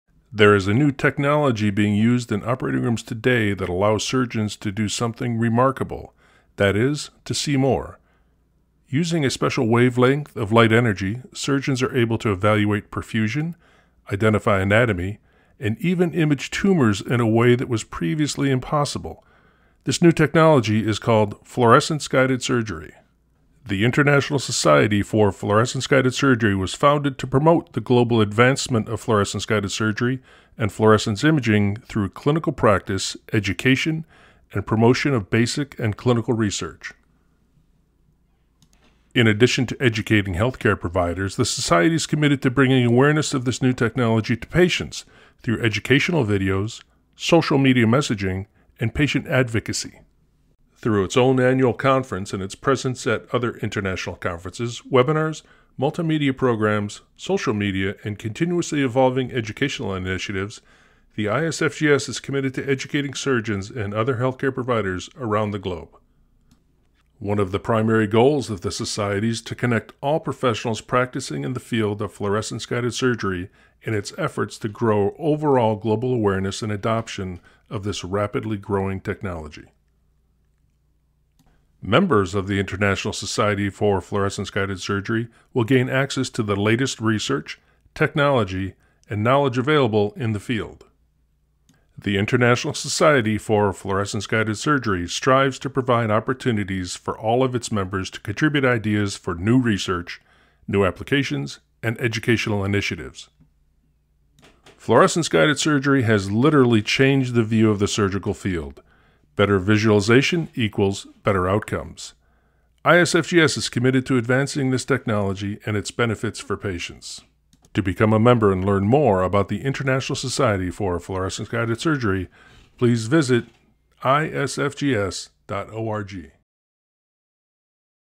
The Leading Society in Revolutionary Surgery. Get to Know ISFGS
The International Society for Fluorescence Guided Surgery is a medical society that advocates and educates in the field of Fluorescence Guided Surgery.

As a revolutionary advancement in medicine, Fluorescence Guided Surgery quickly gained popularity in the medical field. ISFGS produces educational material for physicians, nurses, medical workers and patients. The mission of ISFGS is to help grow the use of FGS globally.